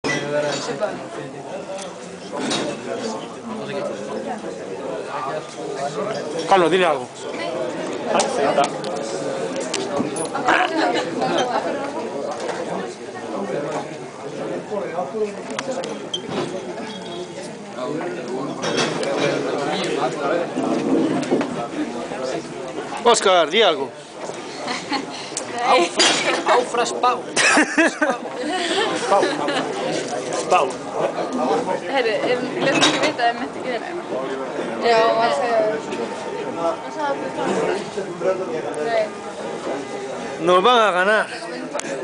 en el palco de autoridades
video grabado en el estadio nacional de Reykjavík durante el transcurso del partido Islandia-España.